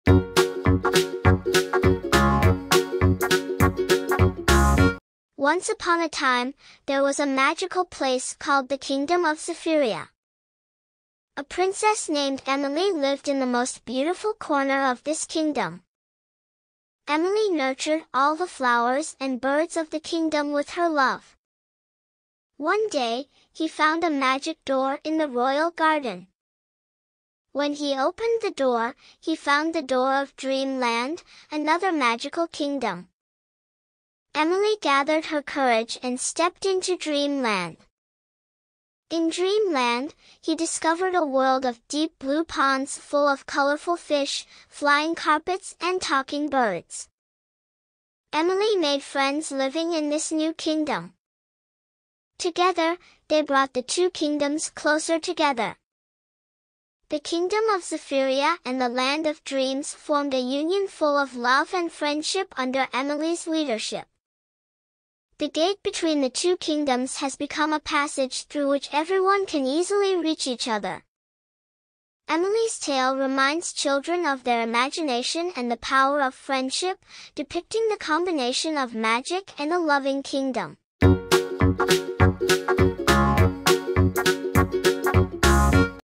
Princess Emily and Her Magical Kingdom 👸🏻 - Daily KIDS Story
✨- Princess Emily and Her Magical Kingdom -✨

Once upon a time, there was a magical place called the Kingdom of Zephyria. A princess named Emily lived in the most beautiful corner of this kingdom. Emily nurtured all the flowers and birds of the kingdom with her love.

One day, she found a magic door in the royal garden. When she opened the door, she found the door of Dream Land, another magical kingdom. Emily gathered her courage and stepped into Dream Land.

In Dream Land, she discovered a world of deep blue ponds full of colorful fish, flying carpets and talking birds. Emily made friends living in this new kingdom. Together, they brought the two kingdoms closer together.

The Kingdom of Zephyria and the Land of Dreams formed a union full of love and friendship under Emily's leadership. The gate between the two kingdoms has become a passage through which everyone can easily reach each other.

Emily's tale reminds children of their imagination and the power of friendship, depicting the combination of magic and a loving kingdom.